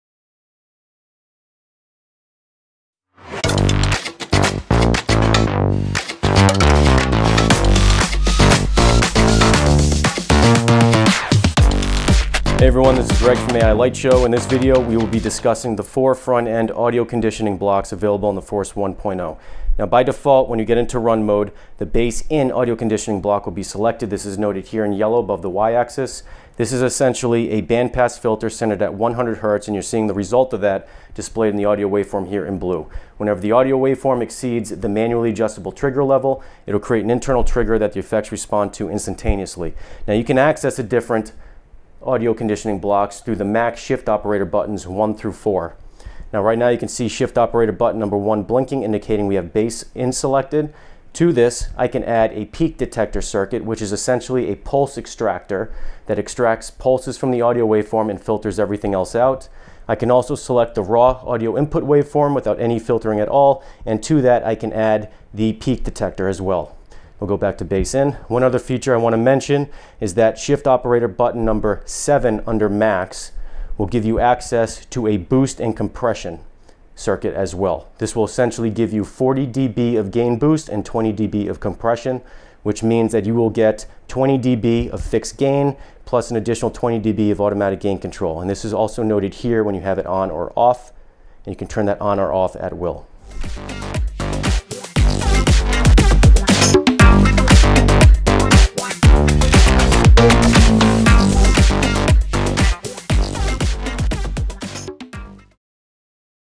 Force 1.0 Audio Conditioning Blocks - Tutorial
There are 4 audio conditioning pipelines available via the Force 1.0:
1.  Band pass filter centered at 100Hz
2.  Band pass filter centered at 100Hz + Pulse Extractor
3.  Raw Audio Input (unfiltered)
4.  Raw Audio Input + Pulse Extractor
The output of the chosen block is what you see via the touch panel display